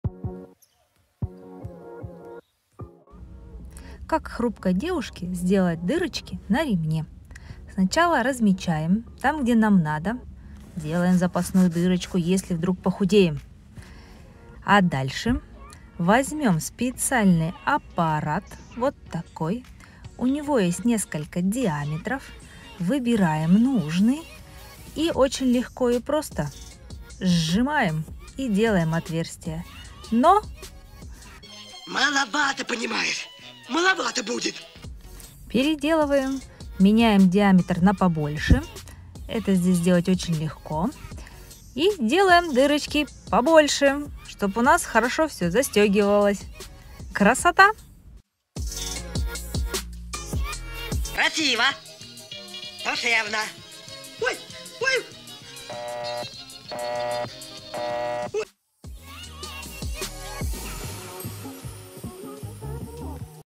Чем сделать ровное и одинаковое отверстие
Продырявит все!

Нужна дополнительная дырочка на ремне? Пожалуйста! Срочно требуется несколько отверстий на сумке? Легко! На картонной заготовке? Запросто!

Для кругового пробойника Prym нет невыполнимых задач, он легко и непринужденно пробивает отверстия 6 различных диаметров (от 2.5 до 5 мм) в самых разных материалах, от кожи и брезента до картона и пленки.

Он удобен, прочен и надежен. И с ним очень легко работать. Смотрите нашу небольшую презентацию  и убедитесь в этом сами! Рекомендовано